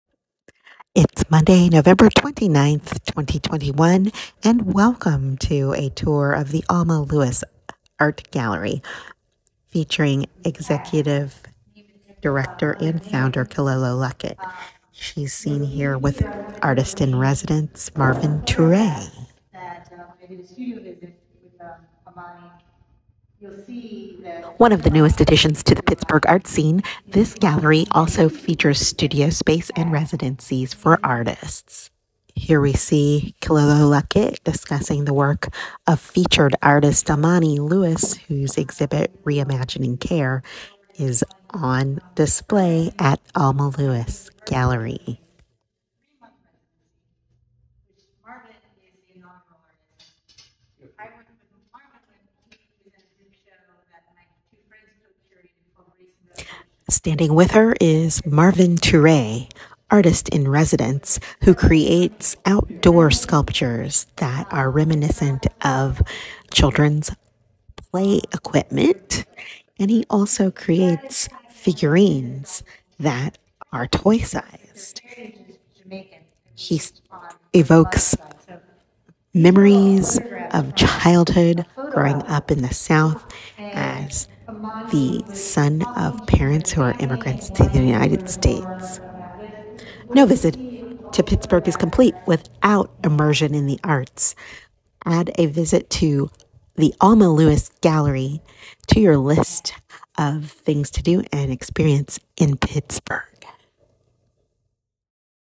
Tour of the Alma | Lewis Art Gallery and Residency
A visit to the Alma | Lewis Art Gallery (@alma_lewis_pgh). We enjoyed this visit to one of Pittsburgh’s newest art galleries and artist residency programs. Featured artist is Amani Lewis. Artist-in-residence is Marvin Touré(@marvintoure).